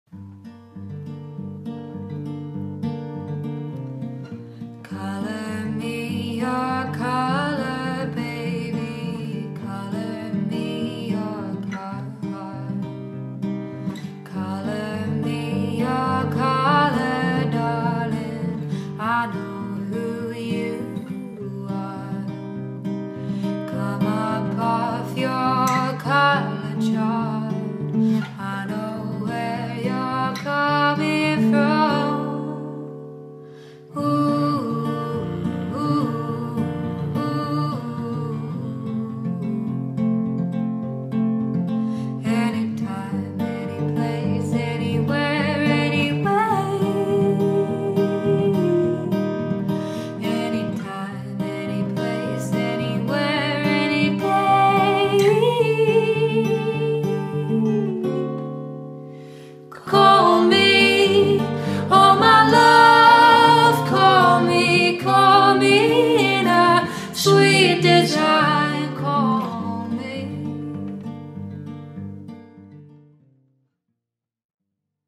SADAG - Searching
It's not always easy to realize that you suffer from depression and for some, the realization comes too late. If you start searching, start speaking.